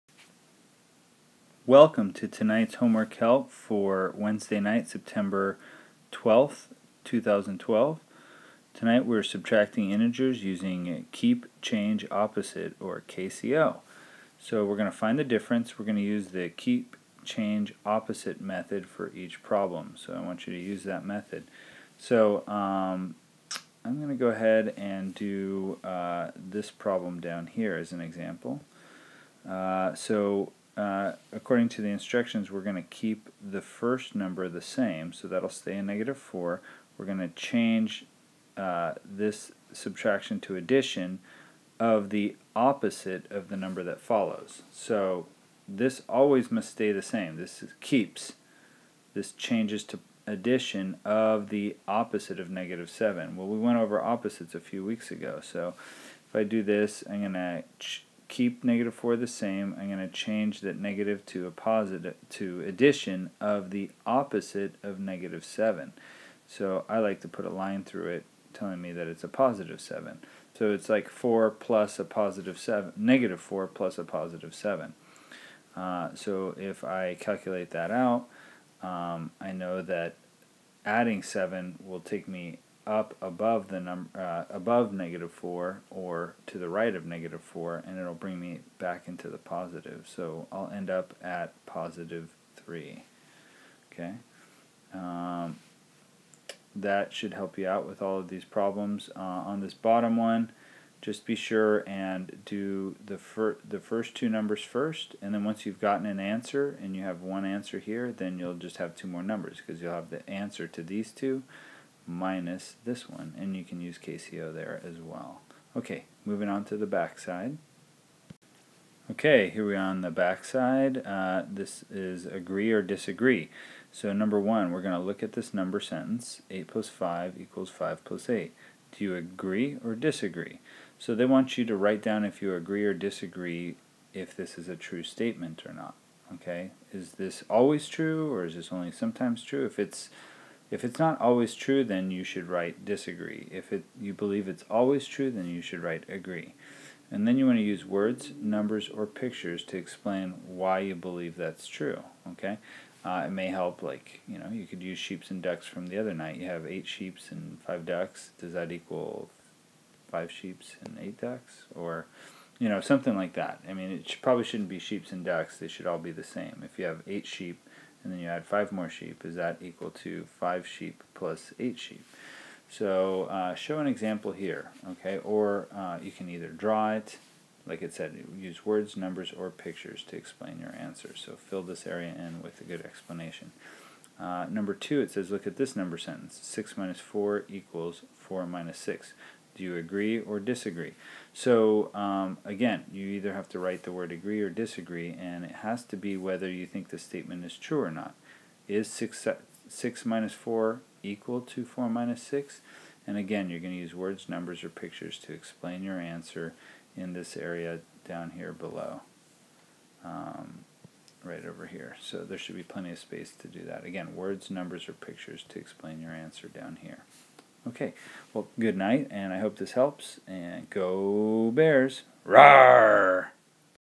HW053 - Integer Subtraction Using KCO / Agree Or Disagree
Pre-Algebra homework help for Burnett Middle Bears for homework HW053 for Wednesday, September 12th, 2012. Go Bears!